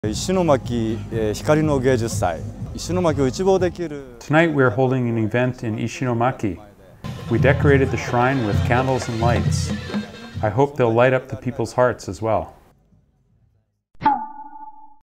020E - Festival of Light No.1 [Ganbatte 365: Follow Japan renewal after Earthquake and Tsunami]
The Ishinomaki Festival of Lights was held in Hiyori Park, Ishinomaki in Miyagi Prefecture. Demonstrations of turning plastic back into energy teach youngsters sustainability as well as lessons of recovery. Special music and dance performances complemented the symphony of lights. Everything from mirror balls to candles to fire dances lighting up the hearts of Ishinomaki people.